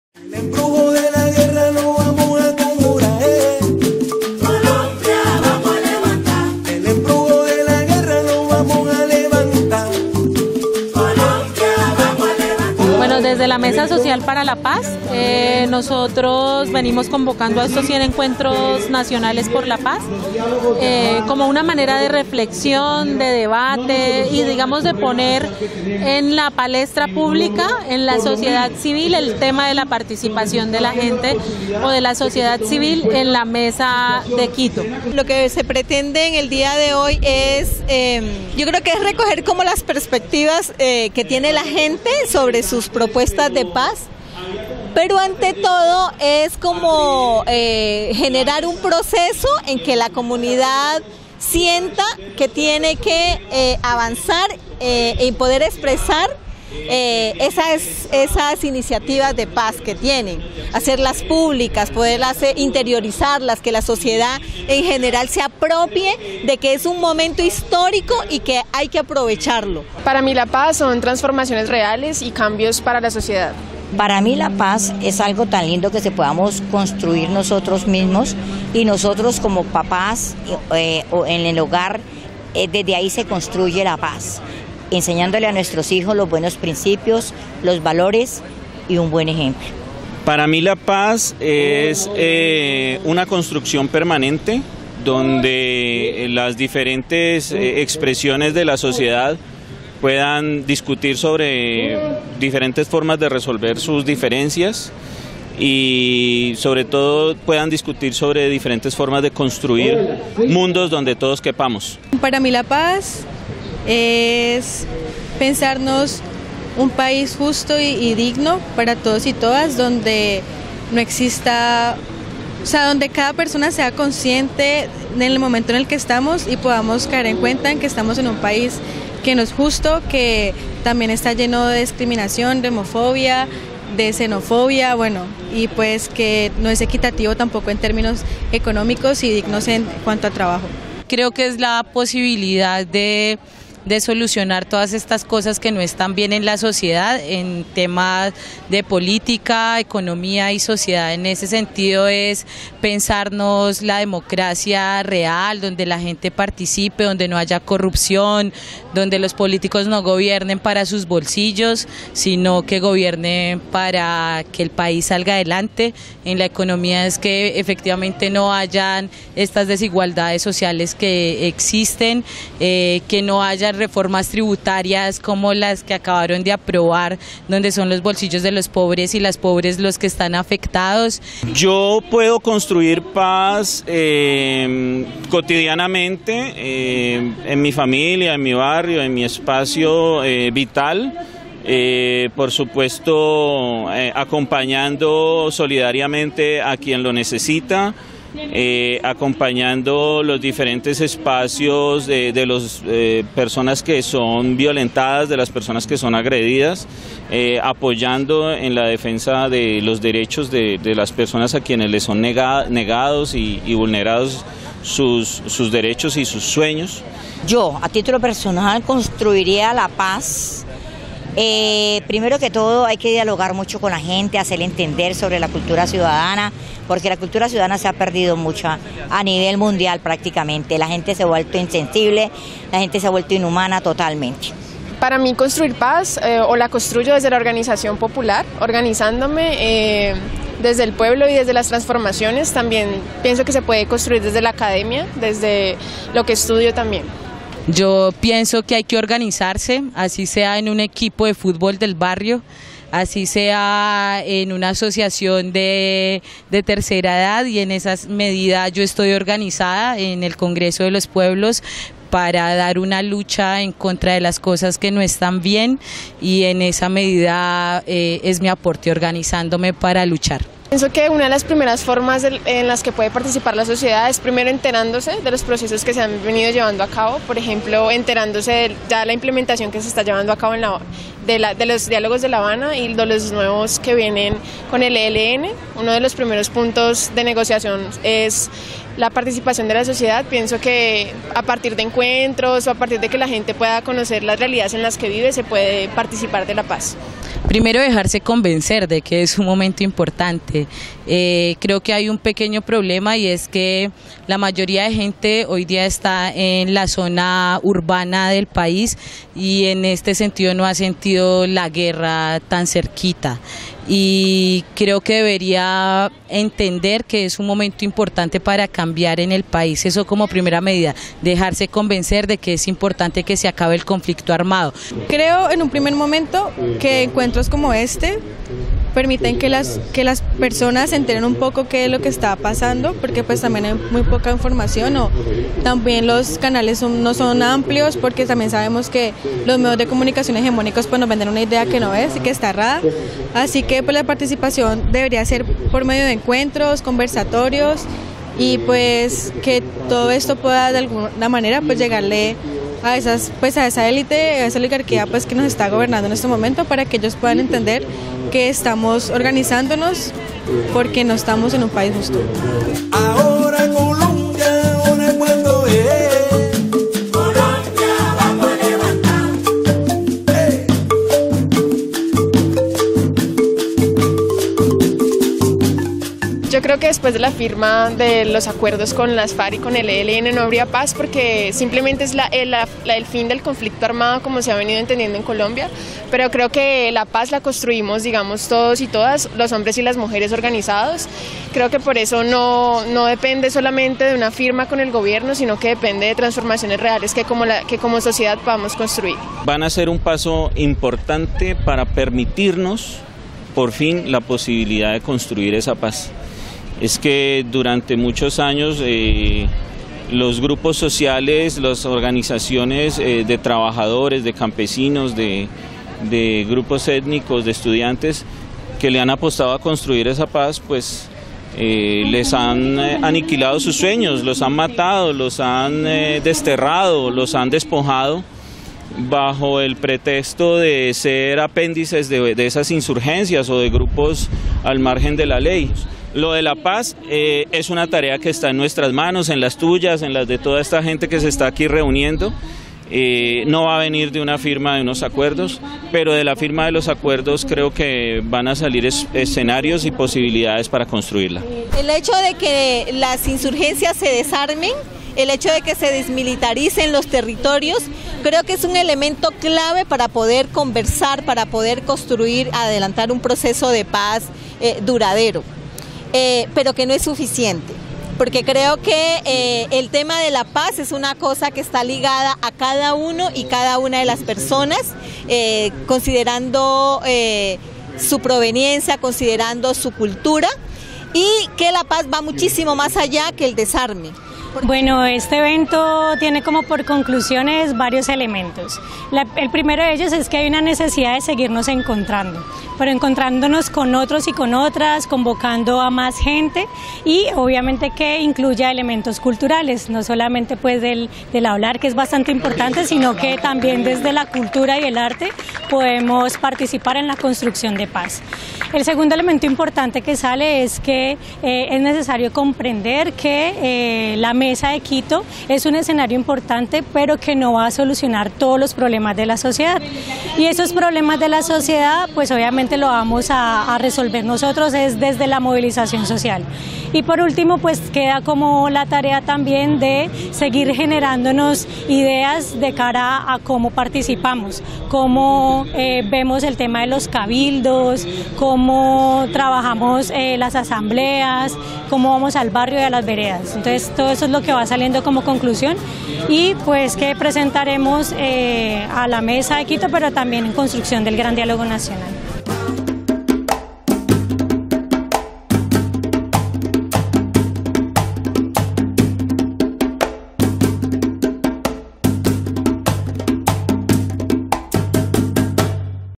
Por una Paz territorial y transformadora, el Valle del Cauca participa
El sábado 4 de febrero en el marco del los 100 encuentros por la paz, organizaciones, pueblos y sectores del Valle del Cauca nos reunimos para seguir el camino de la construcción de un proceso de paz transformadora integral y participativa, en el evento Se escucharon diversas voces que han sido fundamentales en la construcción de paz y sectores que poco a poco se van vinculando.
La paz en Colombia requiere de manera urgente mayores niveles de participación de todo el pueblo colombiano. Se necesita analizar, evaluar y proponer salidas a la crisis social, económica, política y de derechos humanos  que se afronta en la actualidad. Desde la Ciudad de Cali nos sumamos hoy para avanzar hacia el Gran dialogo Nacional y nutrir con propuestas concretas el momento histórico que vive el país.
 Y sectores que poco a poco se han venido vinculando en la construcción de este proceso.
Convocar y convocarnos para proponer, escuchar y resolver, impulsar encuentros, movilizaciones y acciones publicas de participación, seguir construyendo propuestas para lograr las transformaciones sociales estructurales y garantías de no repetición. La acción participativa del pueblo colombiano en su conjunto, requiere consultar y consultarnos, escuchar la voz de las víctimas del conflicto social y armado. Sabemos que la Paz requiere transformaciones sociales estructurales y  acciones reales de voluntad política de los actores del conflicto.
Avanzar participando de manera directa, vinculante y decisoria en la construcción de paz.
Ven y tómate la palabra, suma tú vos, háblanos de tus propuestas de paz y comparte con nosotros :

¿Porque crees que requerimos mayores niveles de participación?
¿Qué piensas de los diálogos de paz? ¿Has participado de ellos?
¿Crees que el pueblo colombiano debe participar en el gran dialogo Nacional por la Paz?
¿Crees que se requiere analizar, evaluar y proponer salidas a la crisis social, económica y política que se afronta en la actualidad?
¿Cómo crees que se pueden lograr importantes niveles de verdad, la justicia y  reparación integral?